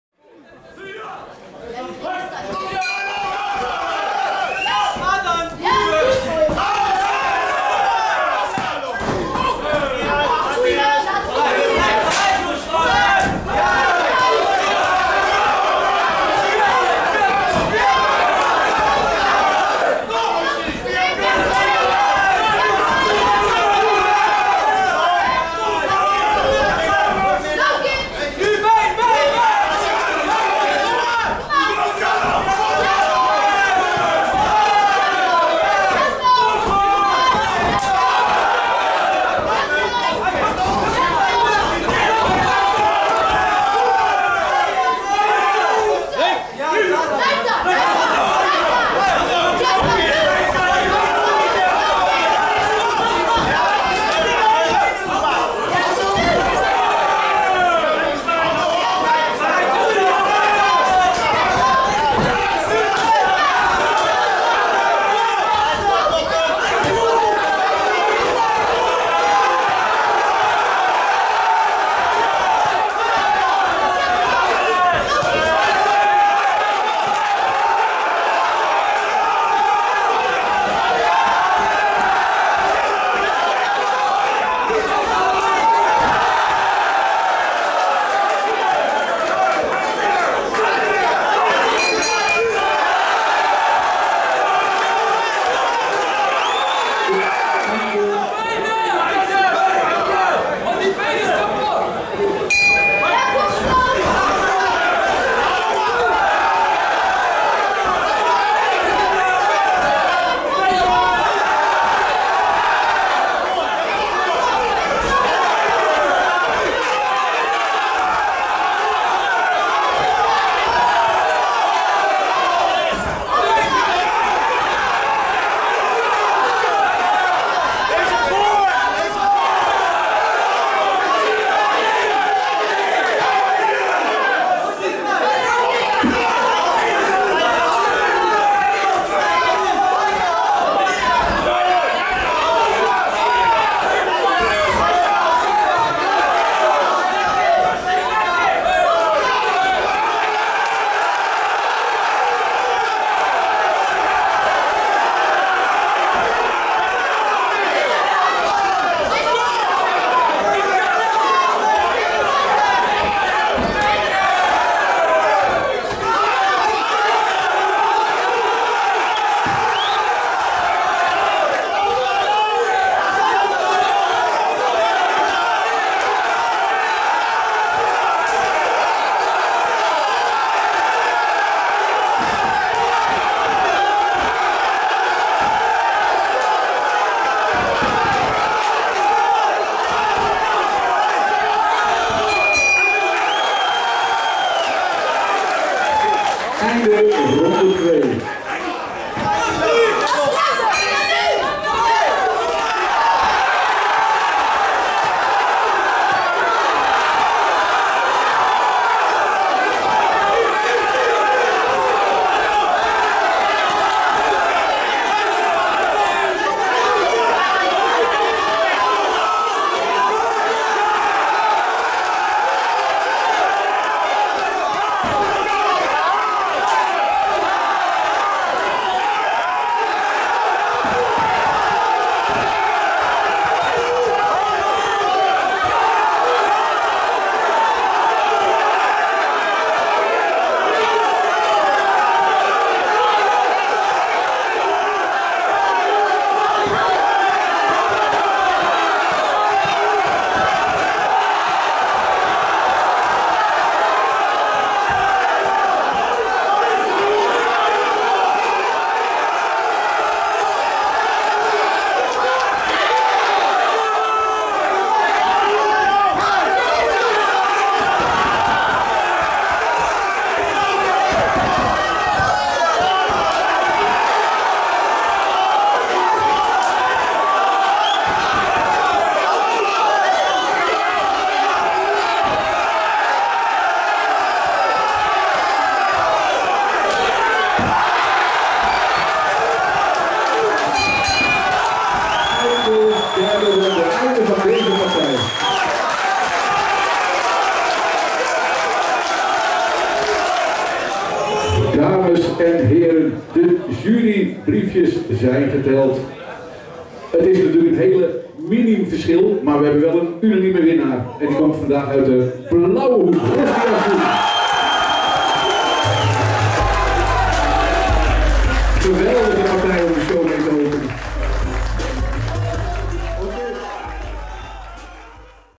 Carlo Heijkamp vs Rochdi Azoum
Next Generation Warriors #14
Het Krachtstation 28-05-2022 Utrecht